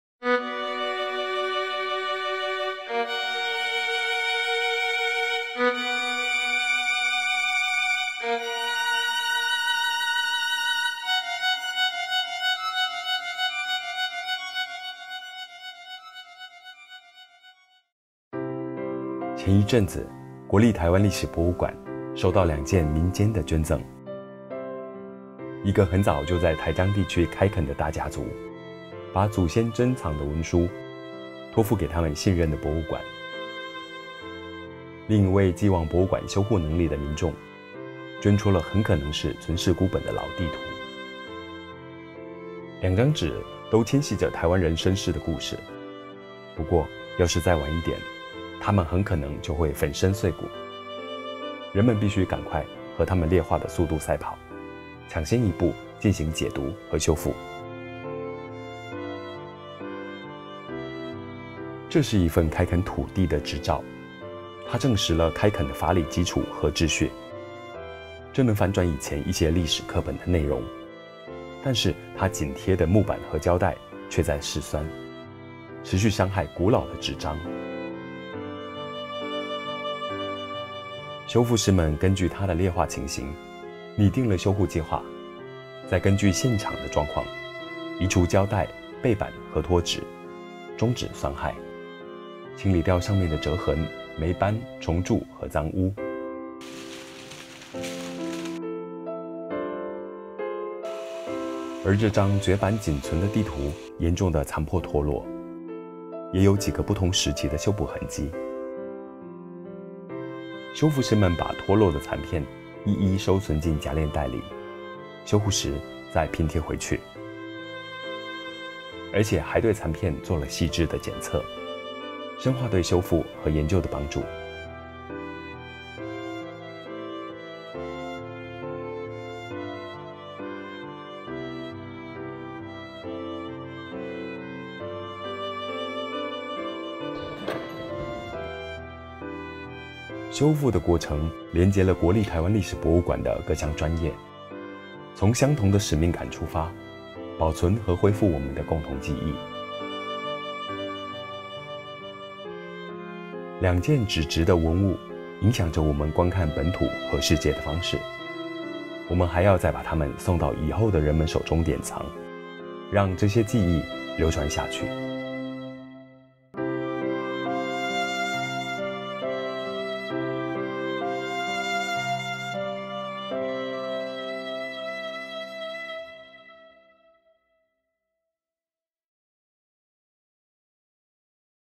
紙上的記憶 The Memory on Paper
臺史博內有一類占館藏大宗的文物類別：紙張，
因為紙張應用的普及，
舉凡從地契、手稿、地圖、水彩繪畫、獎狀、書籍、日記、照片等等，
它們的基底材都是紙質，
臺灣氣候的高溫高溼，或是頻繁使用造成裂痕，
常會有佈滿各式膠帶和黴菌的紙質類文物，
隨著時間的推移，膠帶上的黏膠將滲入紙張纖維內，
進一步還會造成紙張脆化，
因此這類型的紙張都是「4級狀況」（需緊急修護），
膠帶移除後再將它們替換成修護用皮料紙，並以純小麥澱粉糊進行修護工作。
這些細膩的修復步驟用文字可能不易傳達，讓我們來看看短片吧！